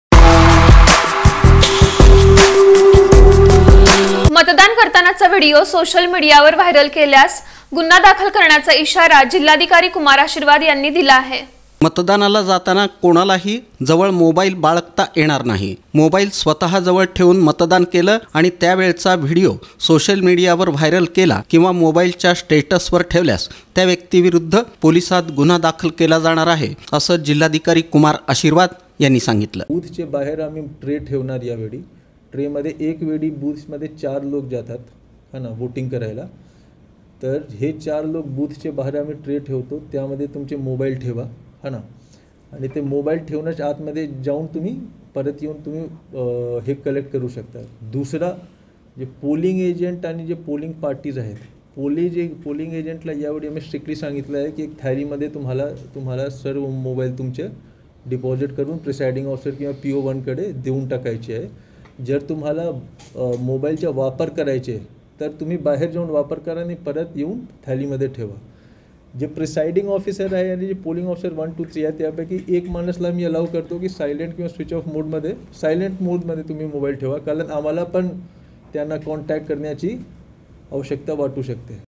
मतदानाचा व्हिडीओ व्हायरल नको... केल्यास गुन्हा दाखल होणार... | MATDAN VIDEO VAIRAL GUNHA
मतदानाचा व्हिडीओ व्हायरल नको... केल्यास गुन्हा दाखल होणार...